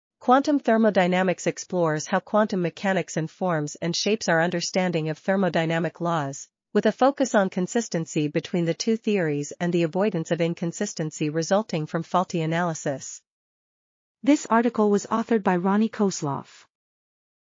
Quantum Thermodynamics: A Dynamical Viewpoint | RTCL.TV
### Keywords ###

### Article Attribution ###
Title: Quantum Thermodynamics: A Dynamical Viewpoint
Authors: Ronnie Kosloff
Publisher: MDPI AG
DOI: 10.3390/e15062100

### Video Timestamps ###
0:00:00 - Summary
0:00:14 - Title
0:00:17 - End